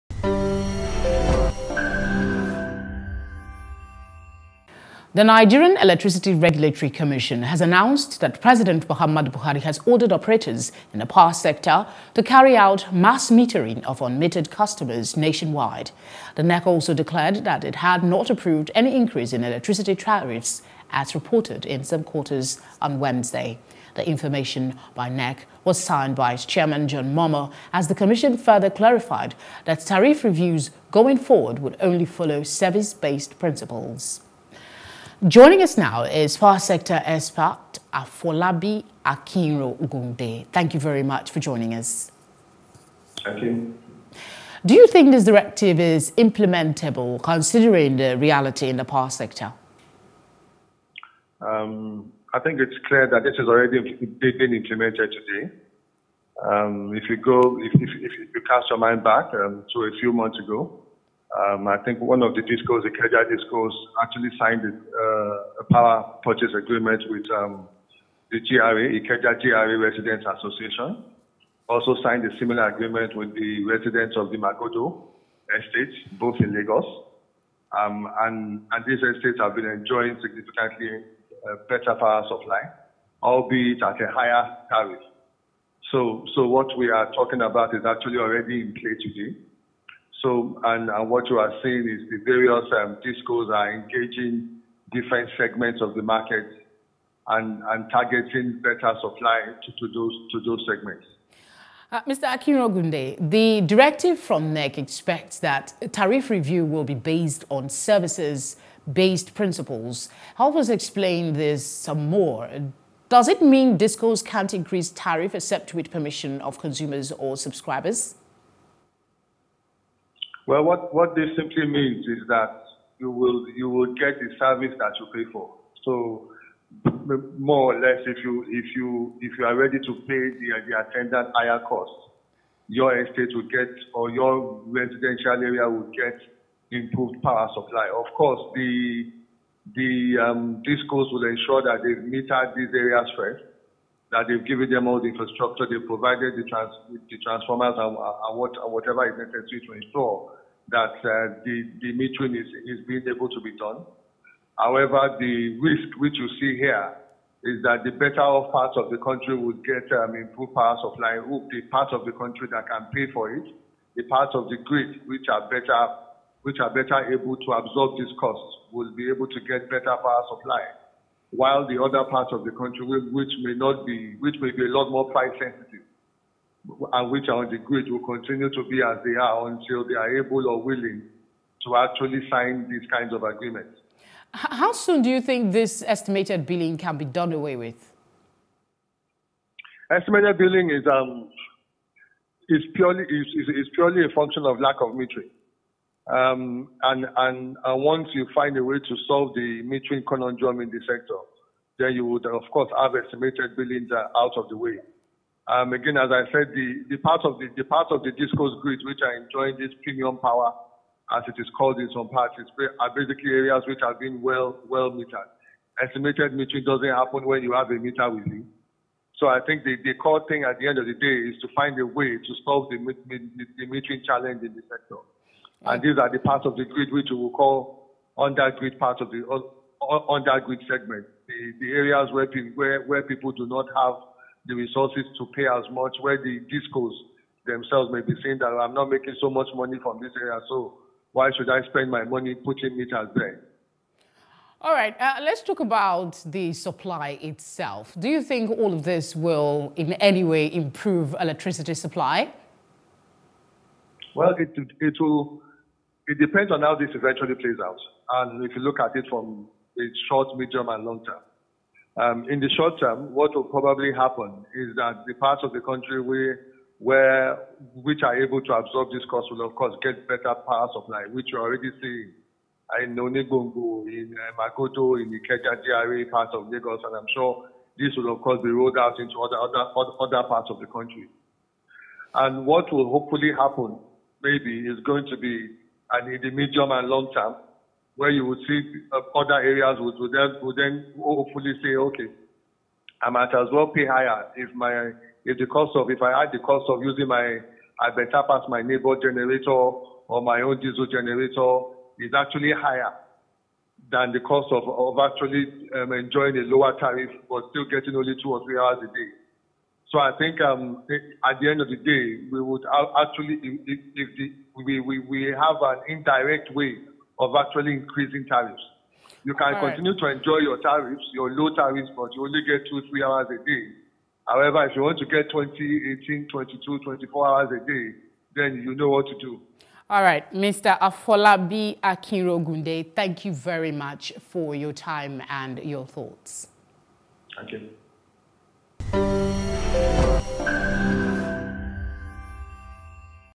No Tariff Increase As Pres. Buhari Ordered Mass Metering – NERC
the Nigerian Electricity Regulatory Commission has announced that President Muhammadu Buhari has ordered operators in the power sector to carry out mass metering of unmetered customers nationwide.